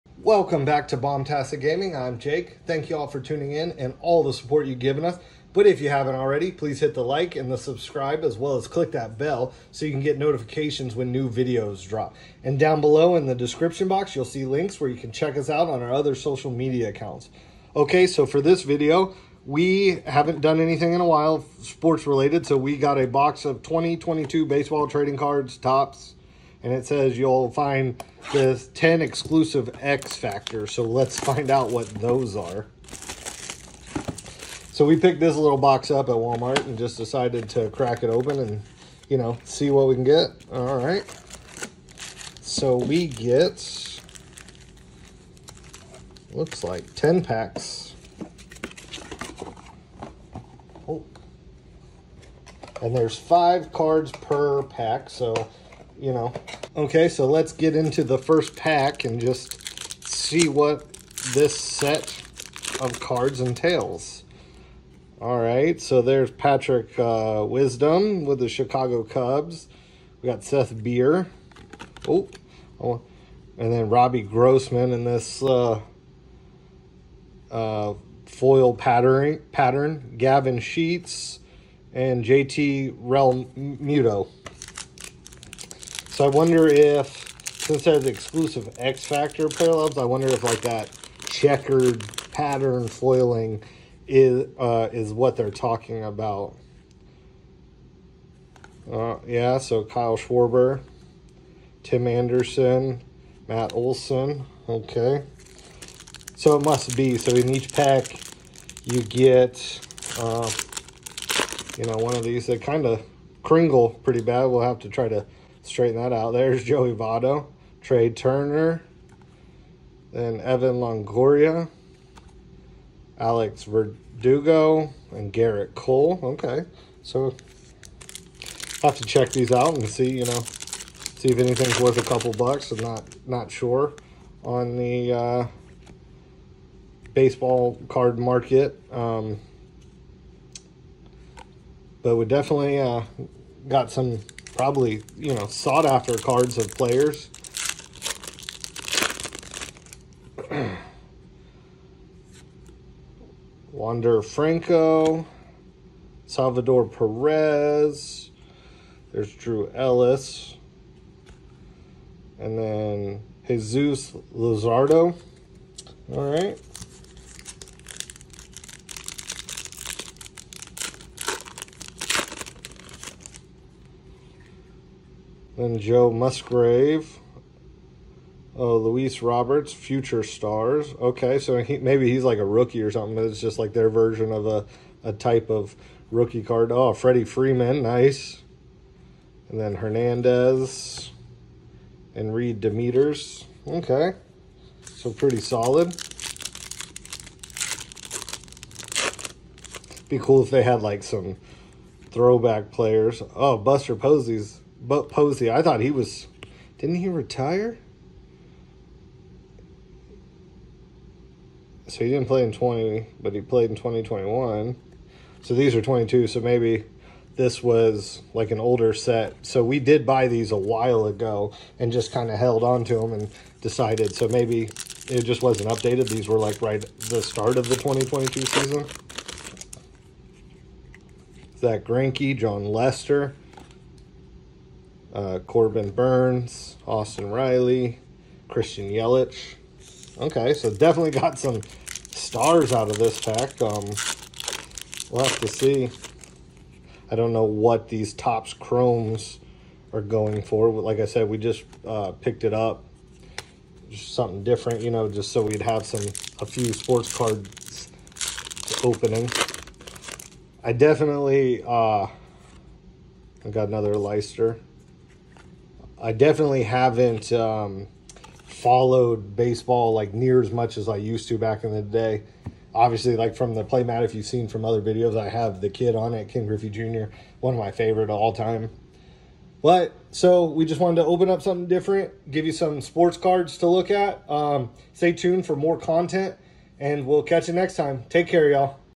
2022 Topps Chrome
Got another Baseball box this time some chrome topps from 2022. Hopefully we get some solid picks!